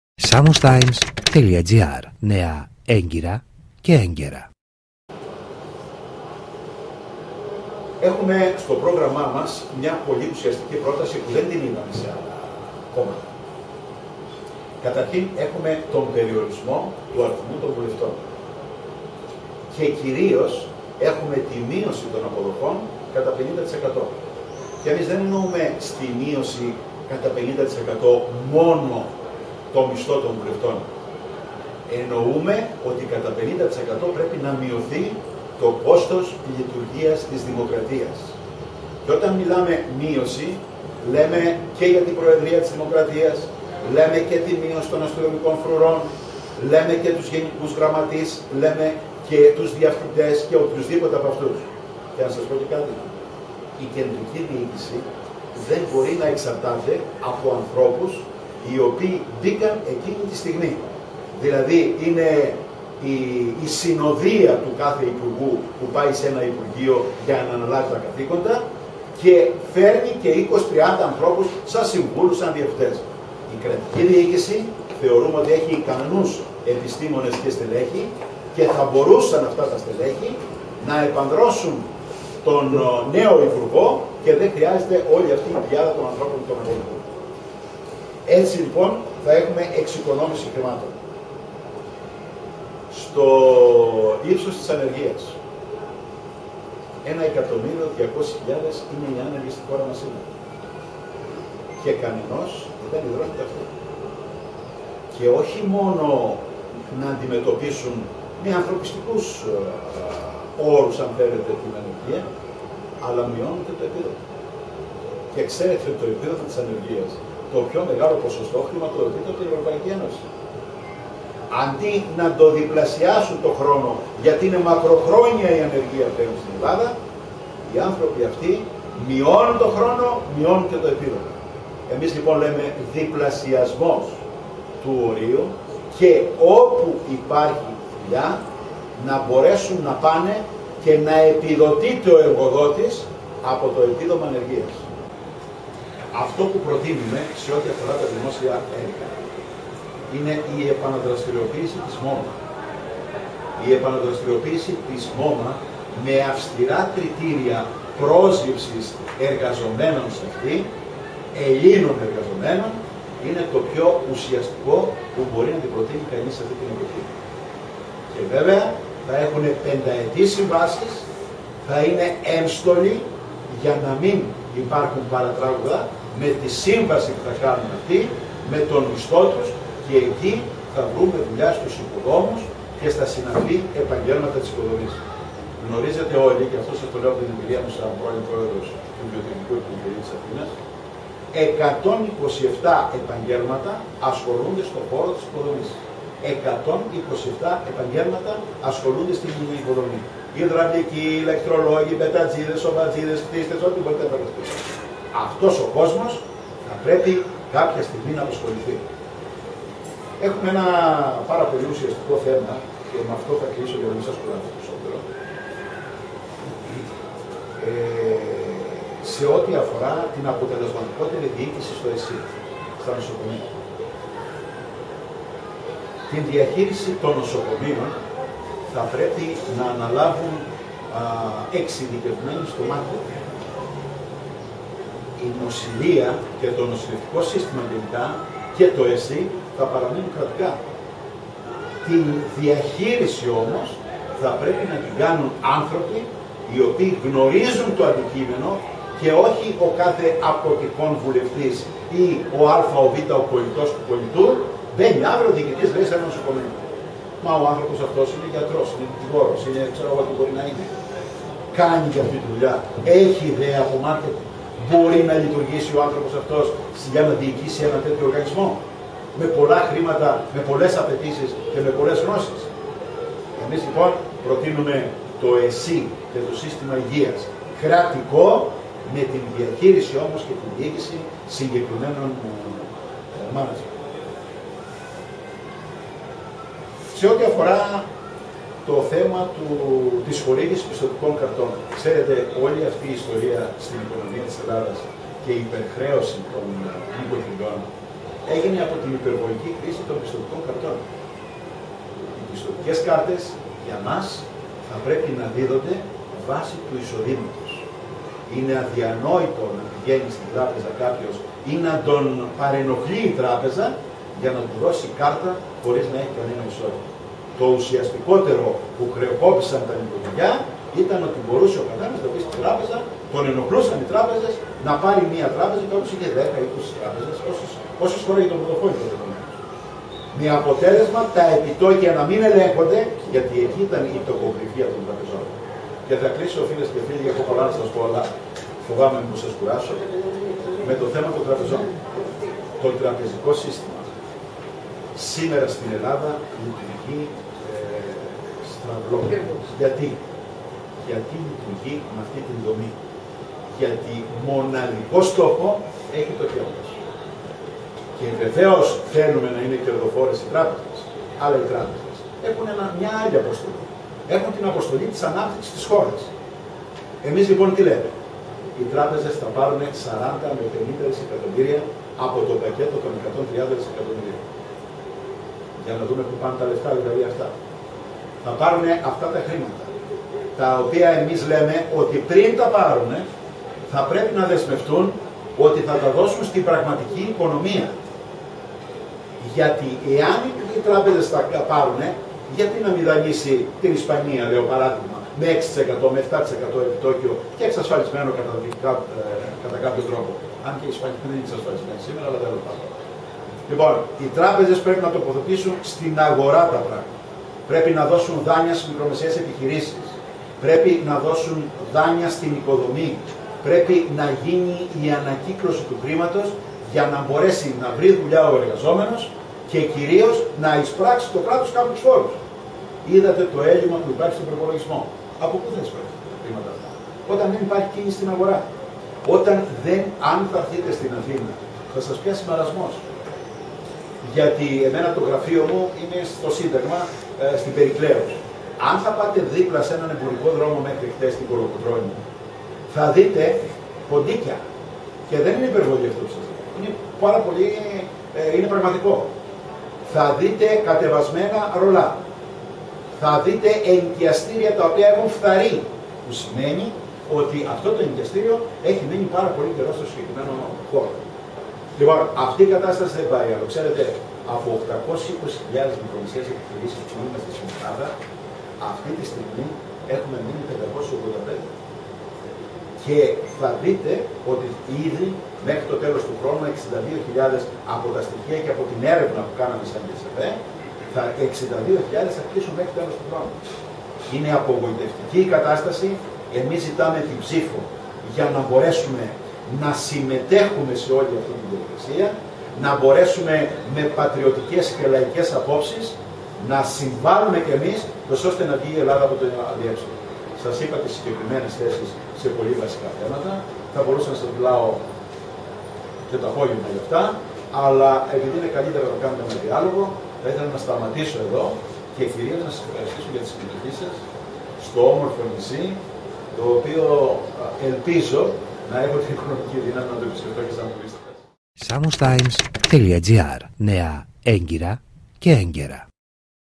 ΓΙΩΡΓΟΣ ΚΑΝΕΛΟΠΟΥΛΟΣ ΛΑΟΣ ΣΑΜΟΣ 9 ΙΟΥΝΙΟΥ
Στη Σάμο βρέθηκε το Σάββατο ο Γραμματέας του Συνδικαλιστικού τομέα του ΛΑ.Ο.Σ. κ. Γιώργος Κανελόπουλος.
Μιλώντας σε συνέντευξη τύπου  στα Μ.Μ.Ε της Σάμου αναφέρθηκε στην τρέχουσα πολιτική ατζέντα καλώντας τον κόσμο να ψηφίσει μαζικά το ΛΑΟΣ στις εκλογές τις 17ης Ιουνίου.
Ο κ. Κανελόπουλος επιτέθηκε προς της Χρυσή Αυγή, τους Ανεξάρτητους Έλληνες και το κόμμα Δημιουργία ξανά του Θάνου Τζίμερου, ενώ αναφέρθηκε και σε ορισμένα σημεία του προγράμματος του ΛΑΟΣ.